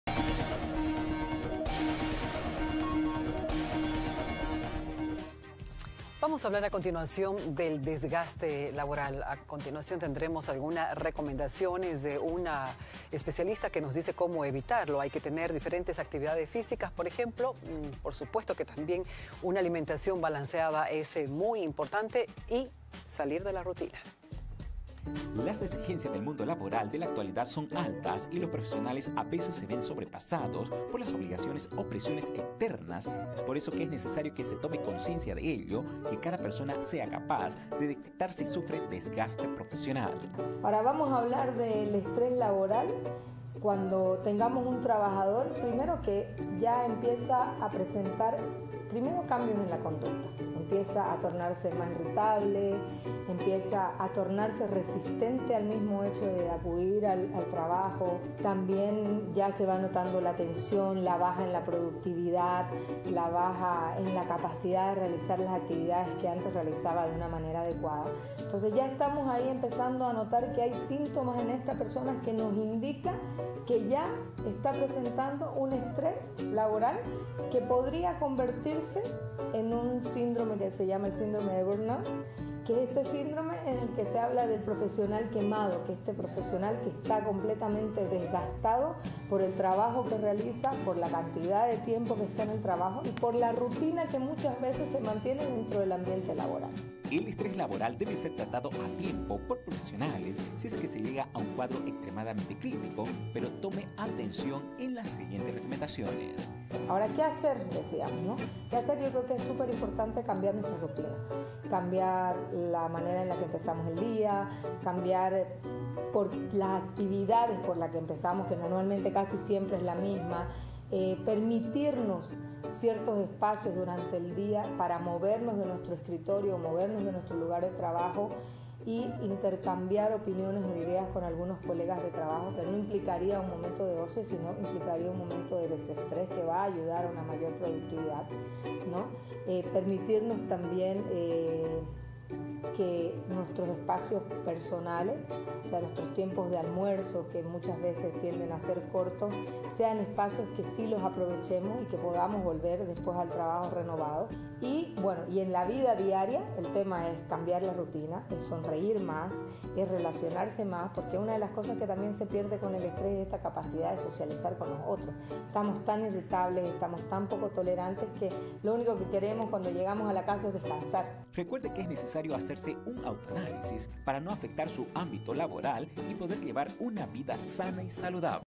Salir de la rutina evita desgaste laboral
Una profesional recomienda que para no llegar al desgaste laboral es necesario salir de la rutina y tener diferentes actividades físicas y una alimentación balanceada.